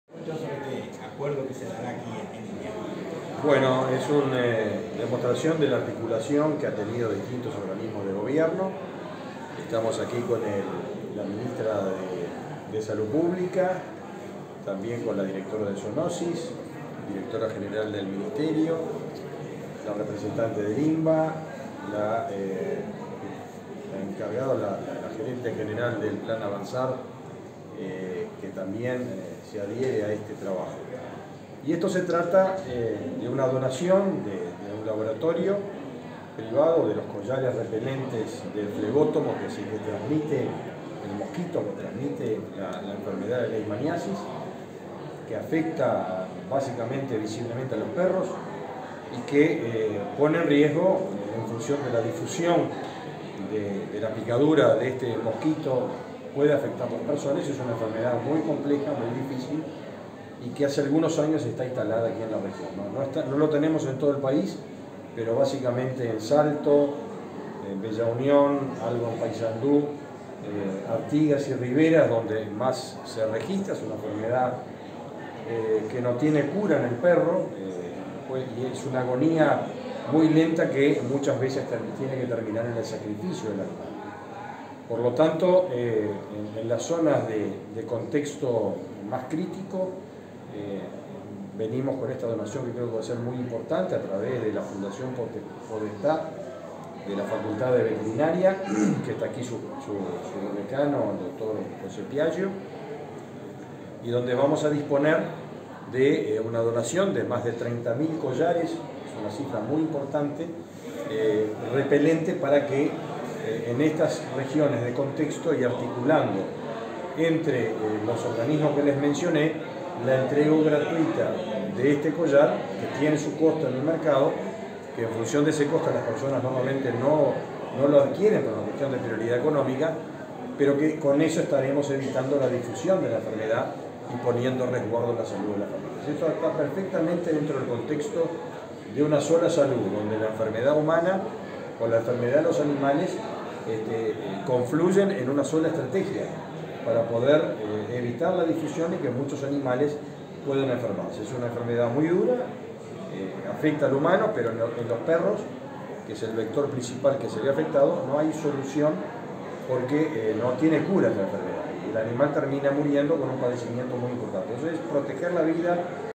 Fernando Mattos: Más de 30.000 collares preventivos de leishmaniasis serán distribuidos
El Ministerio de Ganadería, Agricultura y Pesca (MGAP), el Instituto Nacional de Bienestar Animal (INBA), la Comisión Nacional Honoraria de Zoonosis (CHNZ) y la Fundación Marco Podestá de la Facultad de Veterinaria acordaron la entrega de 30.000 collares para canes en Salto y Bella Unión, a fin de combatir la leishmaniasis. Las instituciones procuran evitar que el vector infectado afecte a los animales e incluso a seres humanos.

Fernando Mattos
El anuncio fue realizado en la mañana de este viernes 1.° en el Instituto Nacional de Investigación Agropecuaria (INIA), en Salto Grande, por parte del ministro de Ganadería, Agricultura y Pesca, Fernando Mattos, la ministra de Salud Pública, Karina Rando, y la directora general del MGAP, Fernanda Maldonado. También asistieron autoridades del INBA y de las demás instituciones involucradas.